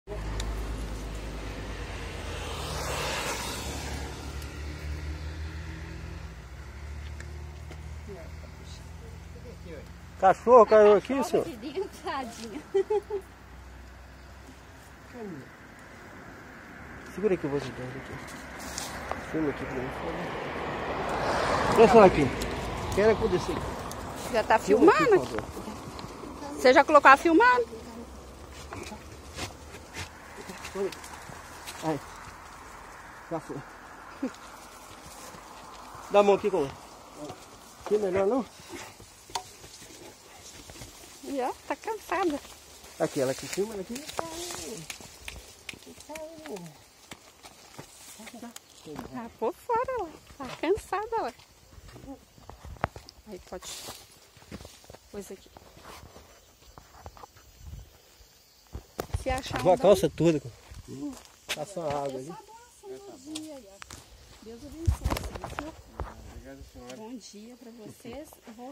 Militares resgatam cadela num bueiro
Uma mulher viu animal caído em buraco de dois metros, à margem da MG-262.